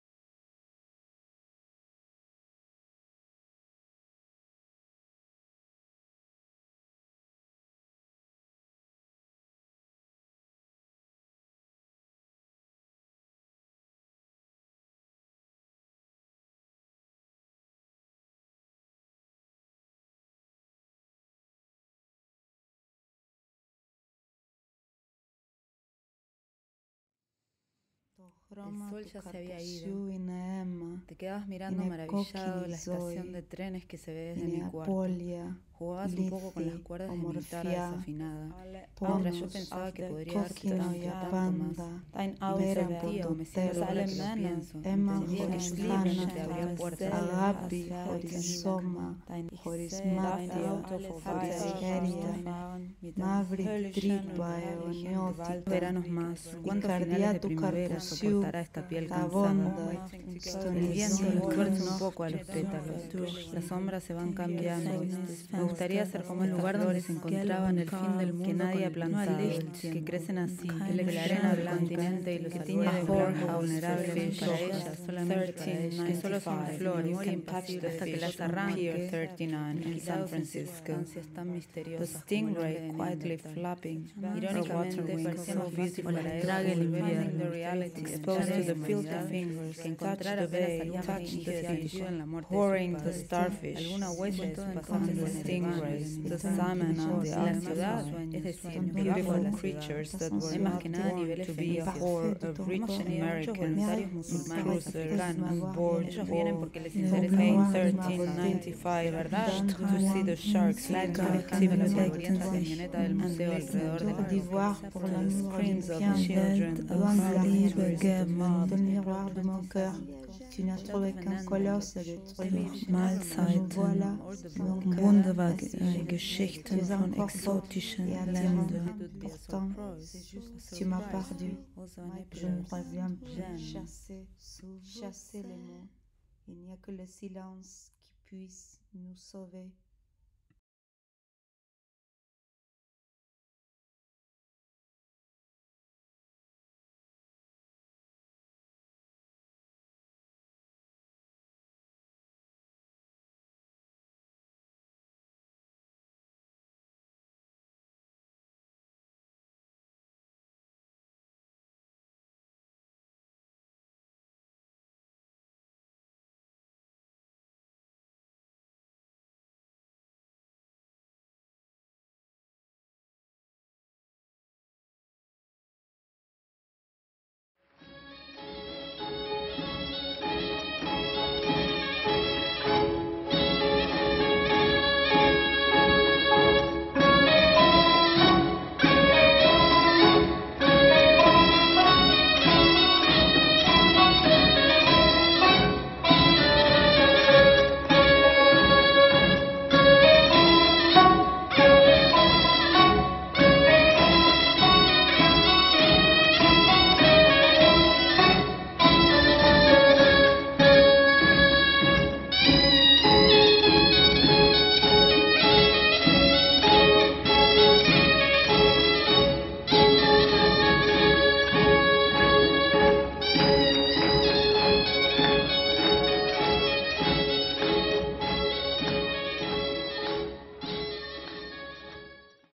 My Marash Documentary WIP
First approach to a documentary about my connection with the Armenian genocide
Developed at FIORD Documentary Workshop 2020
Tutors and presenters included Irina Raffo, Andrés Di Tella, Paz Encina